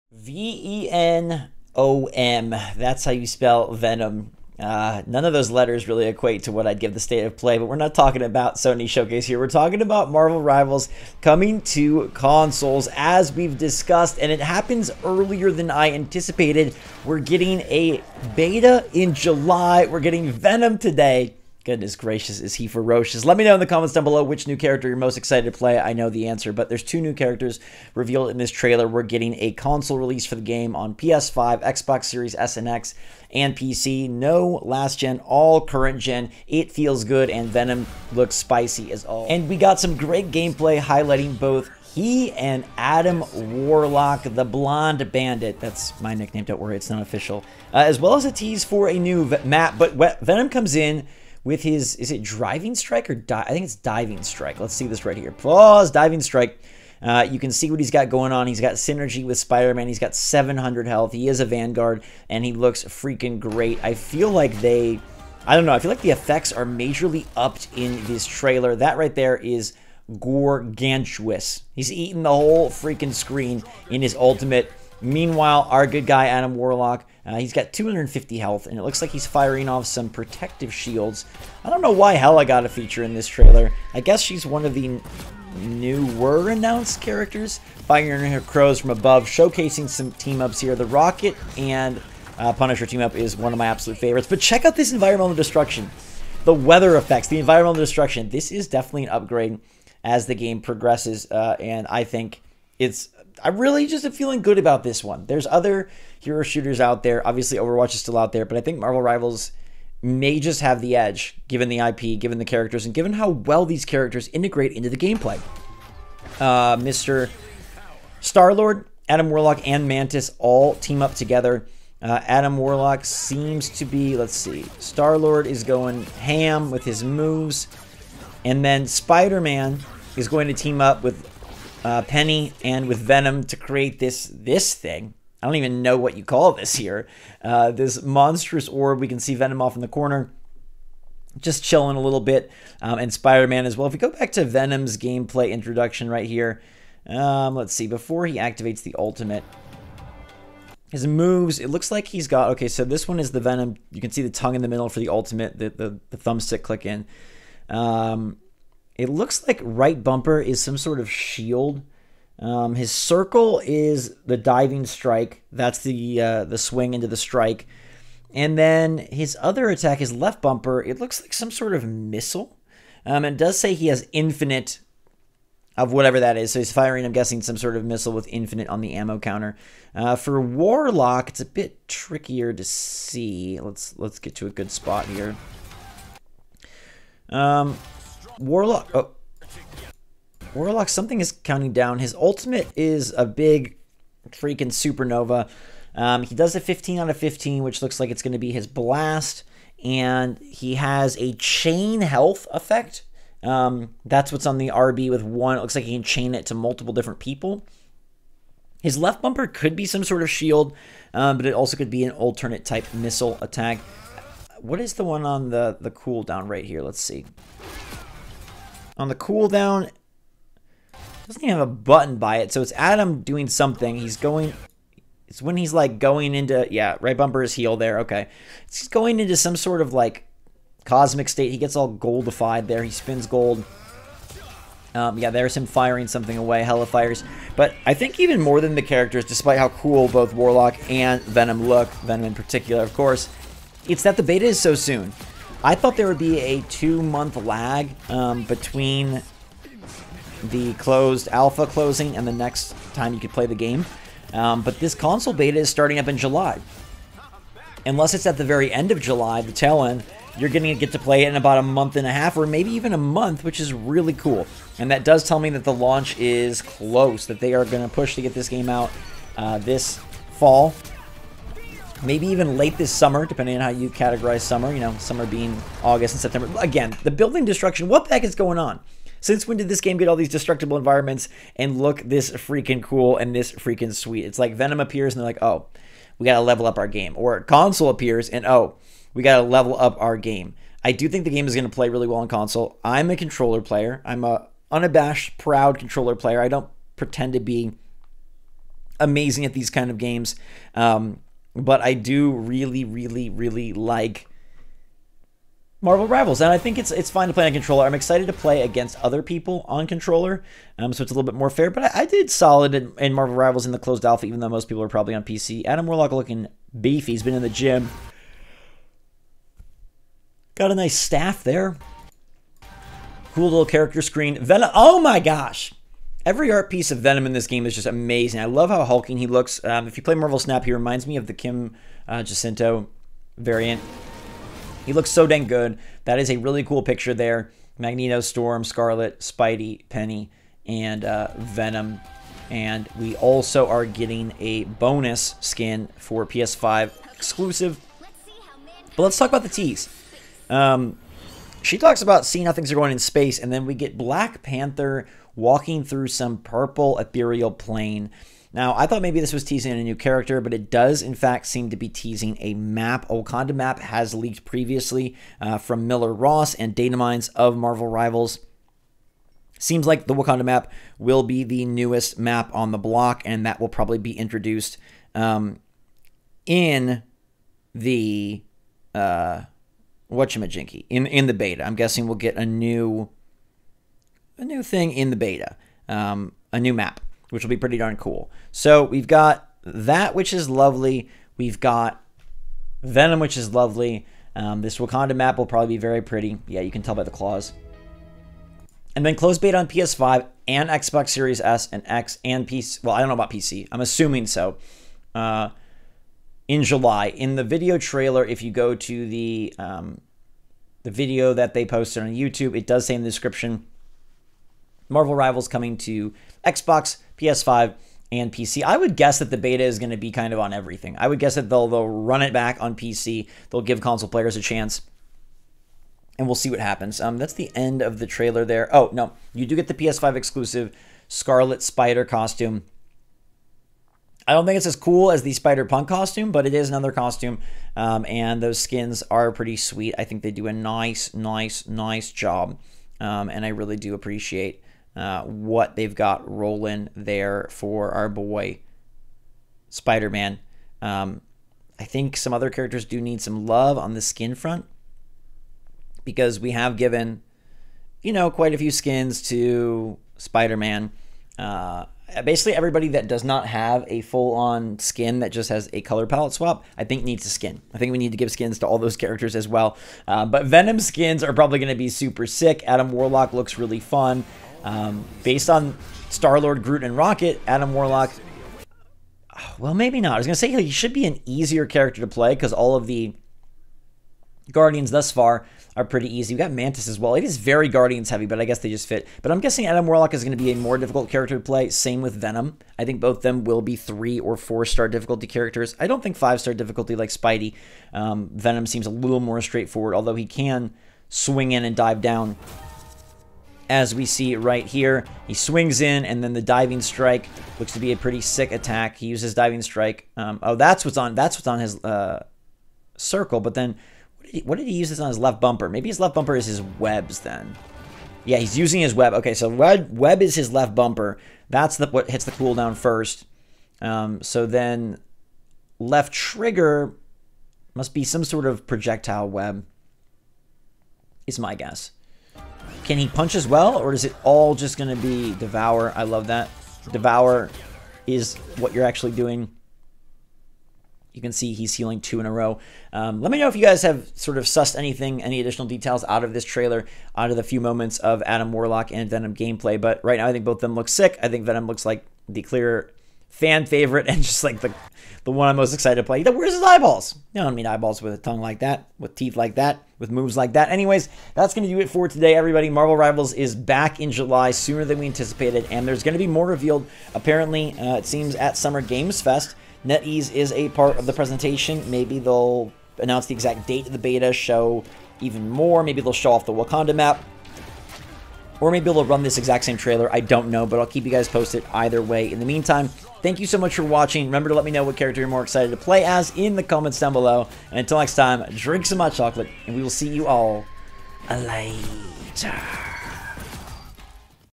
CLOSED BETA CONFIRMED! Marvel Rivals PS5 + Xbox Gets Venom!
Boom! Marvel Rivals Beta coming in July, and so are Venom and Adam Warlock! Who are YOU more excited to play as?